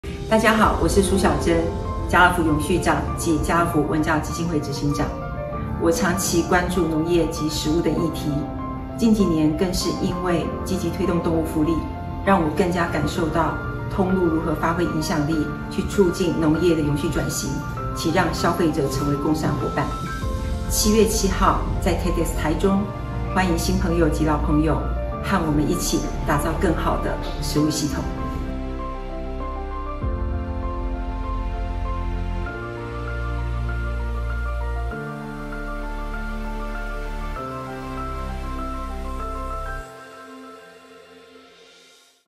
2024年會講者介紹｜蘇小真 Hsiao-Chen Su｜TEDxTaichung
🎉 TEDxTaichung 2024 年度大會《人 SAPIENS》講者介紹

蘇小真 Hsiao-Chen Su
零售業永續轉型先行者
Retail Sustainability Pioneer

蘇小真，動物福利及永續共好的倡議者與實踐者，現任家樂福永續長暨家樂福文教基金會執行長。曾任家樂福行銷總監，擁有政大EMBA及中央大學法文系學位。她以其創新的永續策略為品牌贏得多項獎項，並使個人在2023年榮獲APSAA亞太頂尖永續長獎。作為零售業永續轉型的先行者，她善於結合商業與NGO的力量，透過每年舉辦的文化藝術季推動綠生活轉型，推廣非籠飼雞蛋以促進動物福利，並參與許多地方創生及政府的永續轉型計畫，致力於打造共好生態圈，加速台灣產業永續轉型。

7月7日在裕元花園酒店，期待與你一同「 Be_the_Change 」！